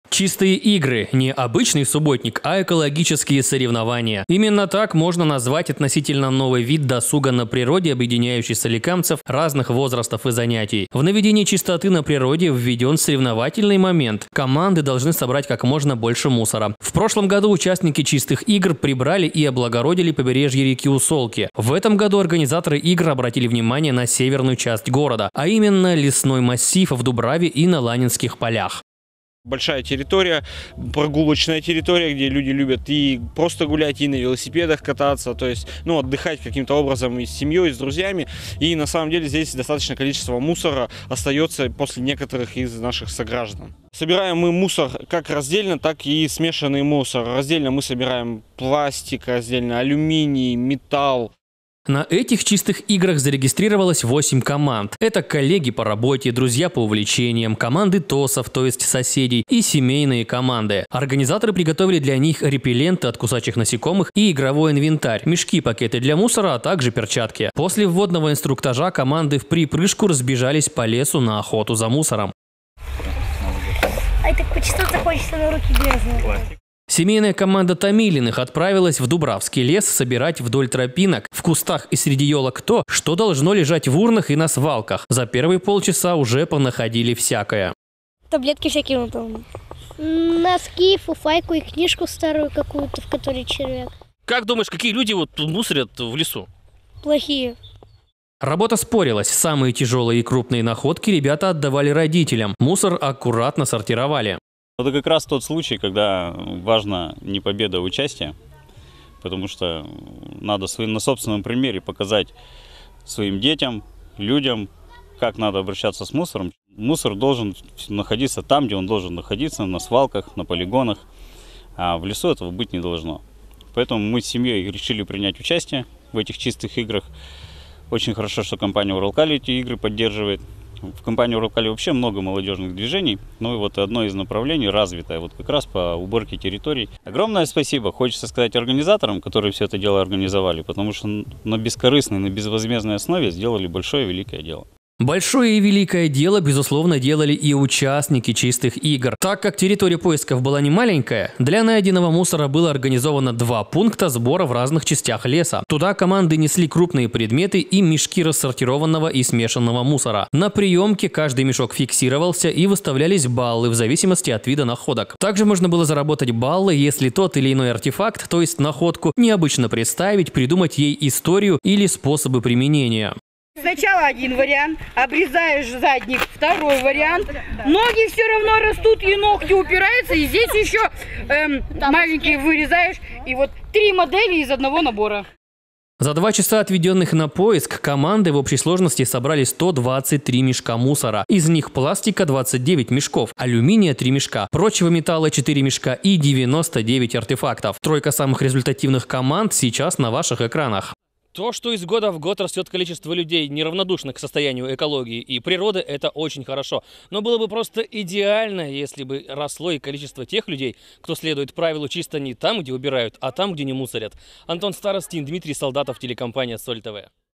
"Чистые игры"_09.07.2018_СольТВ
В минувшие выходные в лесном массиве северной части города проходило интересное соревнование, собравшее около 4х десятков неравнодушных к состоянию природы Соликамцев. Все подробности в материале нашей съёмочной группы.